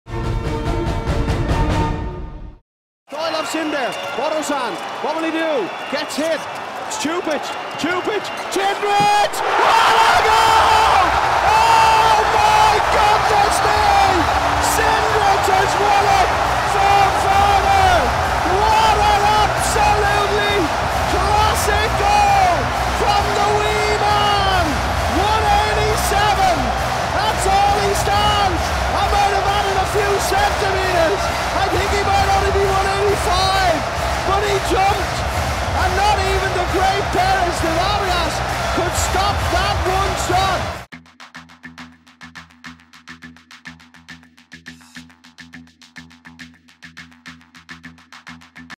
What a goal! Cindric leads Vardar to the final | VELUX EHF FINAL4
It's their first appearance at the VELUX EHF FINAL4 in Cologne and thanks to Luka Cindric's last goal Vardar beat FC Barcelona Lassa 26:25 in the second semi-final.